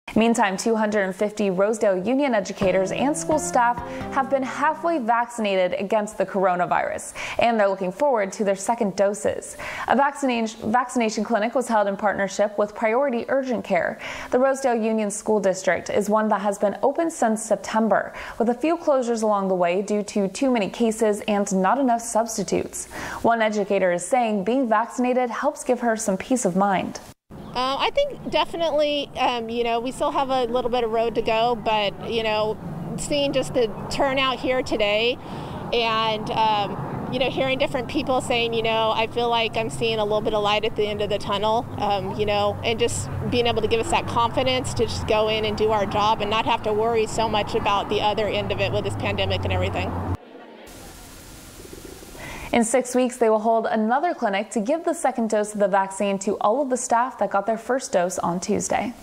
Rosedale Union School District vaccinates 250 educators, staff
Rosedale Union educators and school staff have been halfway vaccinated against the coronavirus and are looking forward to their second doses.